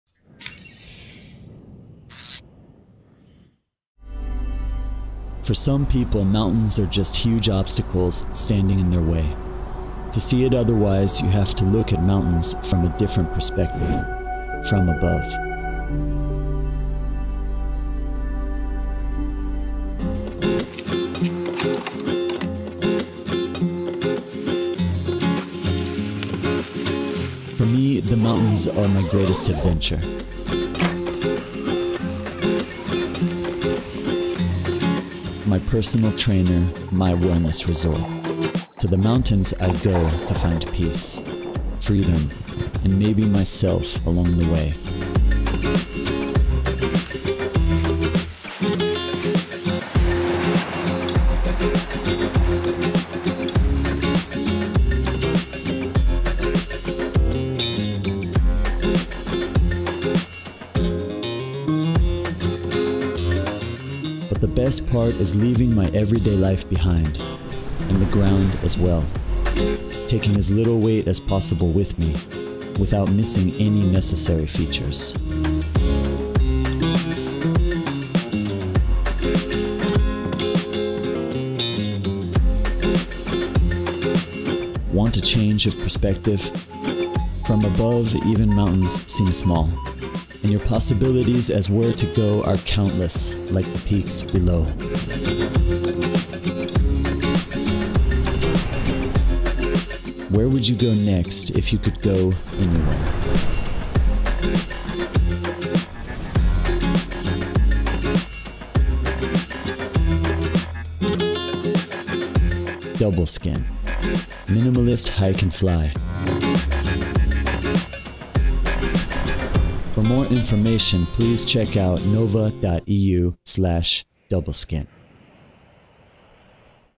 NOVA DOUBLESKIN – minimalist hike & fly wing – the official video
The DOUBLESKIN is a minimalistic hike & fly paraglider. With 2.1 kg at size 17, it is the lightest wing in the world with top and under surface (by same flat surface area, with EN-certification, with the Ultralight Riser; as: 06/19). It packs very small and is a delight in the air thanks to better aerodynamics compared to a single-surface wing. With the DOUBLESKIN walking up is as enjoyable as flying and landing!

Der DOUBLESKIN ist ein minimalistischer Hike & Fly-Gleitschirm und mit 2,1 Kilo bei/ Größe 17 der derzeit der leichteste Schirm der Welt mit Ober- und Untersegel (bei gleicher ausgelegter Fläche, mit EN-Zulassung, bei Wahl der leichten Tragegurtvariante; Stand 6/19). Der Doubleskin ist winzig klein packbar und in der Luft die reine Freude dank der besseren Aerodynamik im Vergleich zu Single-Skins. So macht der unbeschwerte Aufstieg genauso Spaß, wie der Flug inklusive Landung.